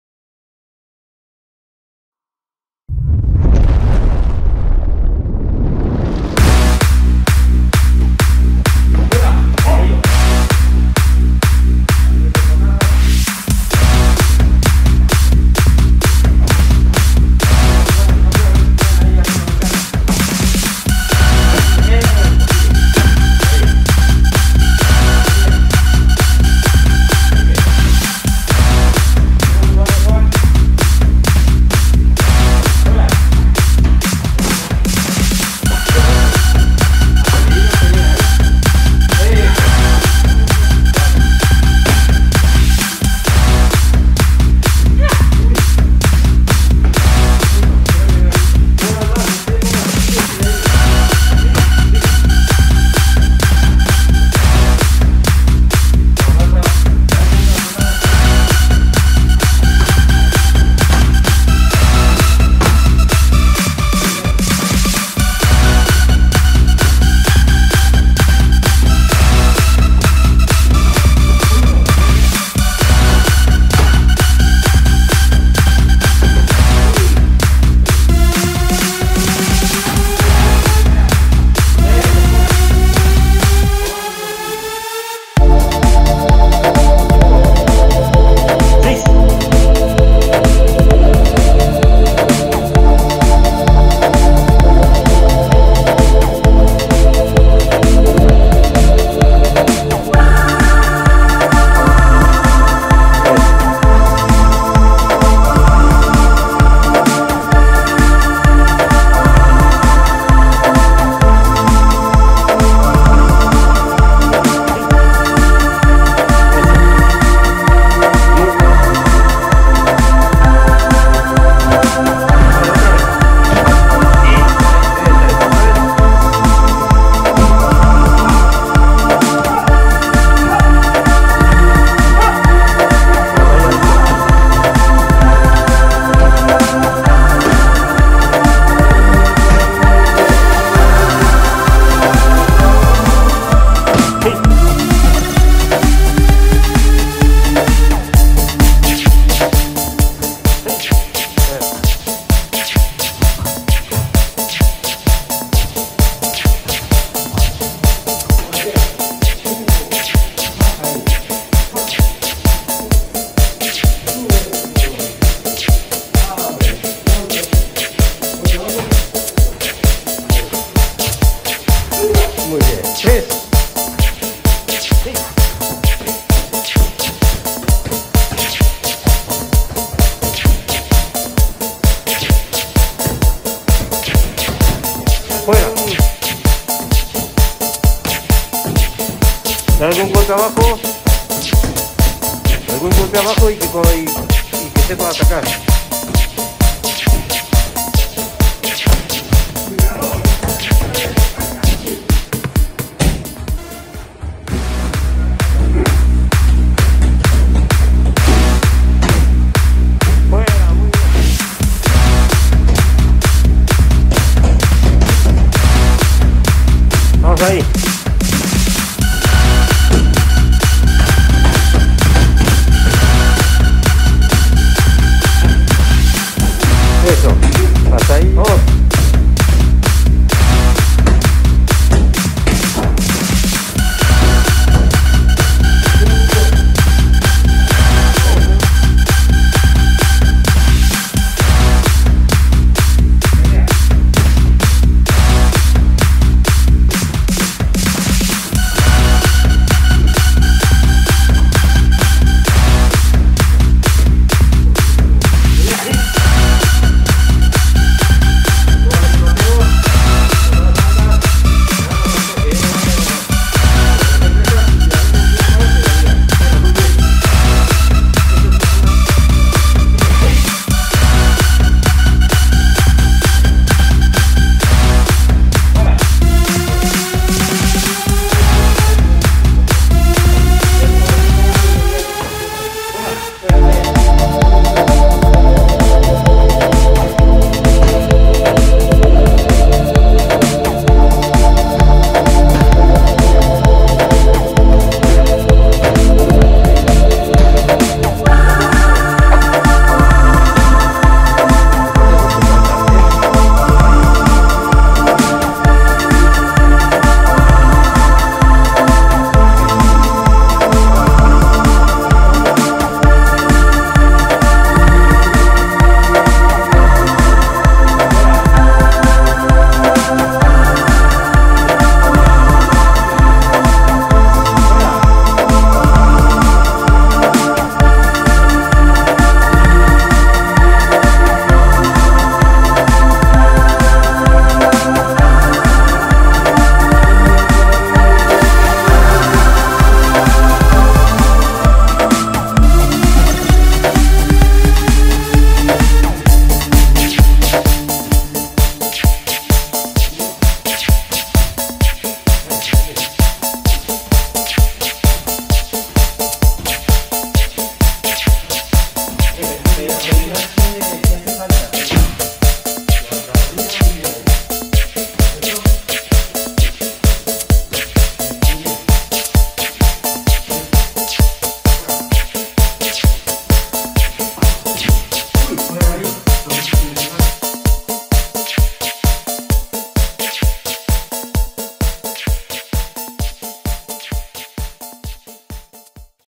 Resumen entrenamientos Febrero 2022 - Limalama
Limalama - La última defensa Madrid.
Resumen de entrenamientos, practicando combato marcando únicamente, técnicas de defensa personal y pateo.